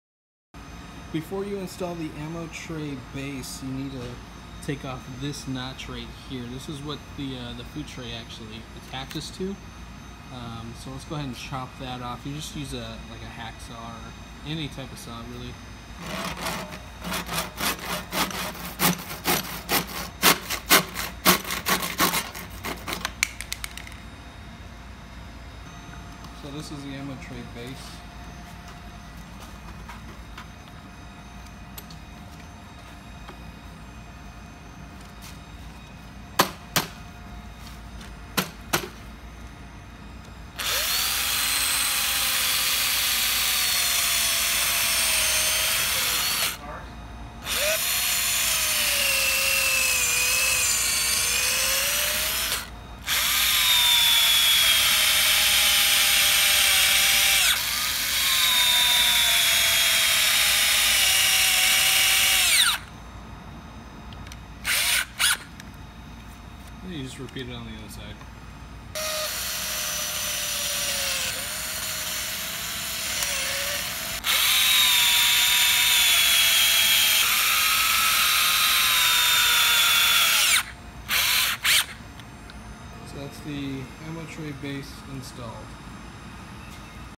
Attaching Ammo Tray Base
A short video on attaching the ammo tray base on the gunstroller.